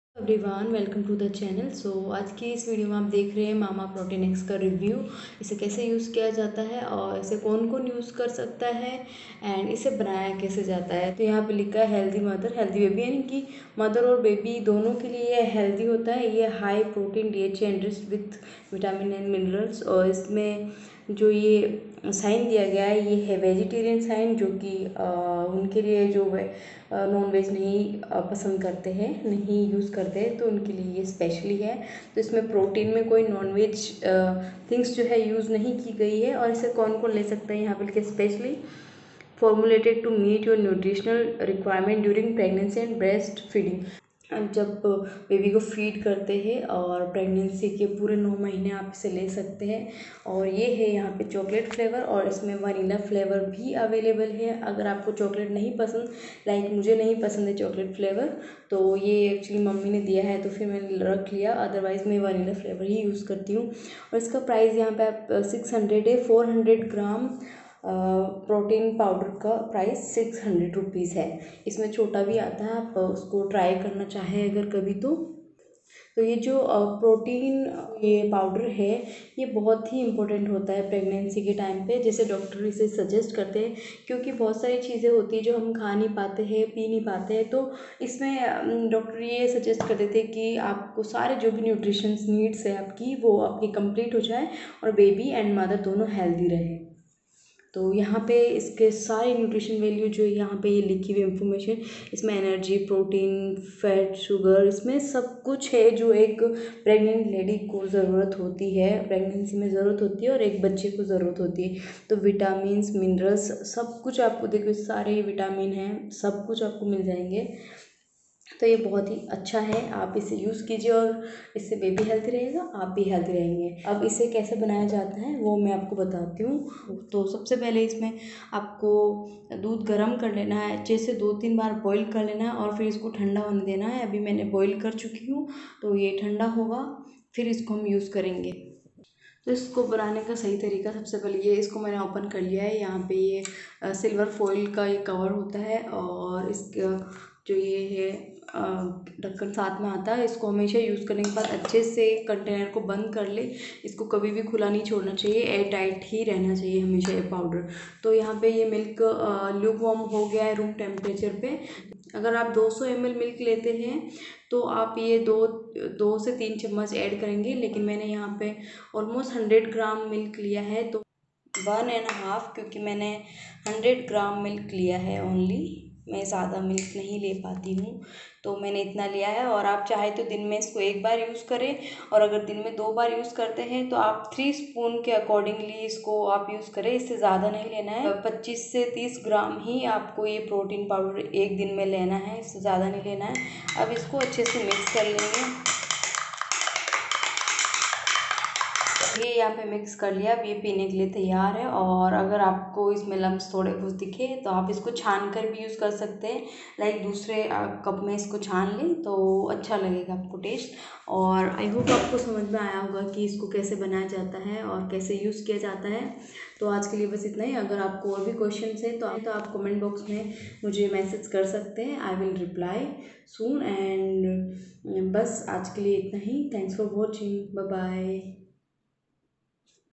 Protein powder | Mama Protinex के फायदे | Protinex mama review
Mama Protinex supplement for pregnancy | Cover Morning Sickness & Breast feeding
Some time women feel morning sickness and weakness during pregnancy so it will help to reduce such type of problems and It also help to increase breast feeding because this product help on it. The lady who is 15 weeks pregnant then she ( pregnant women ) take easily with doctor concern . It would be nice for best supplement in pregnancy.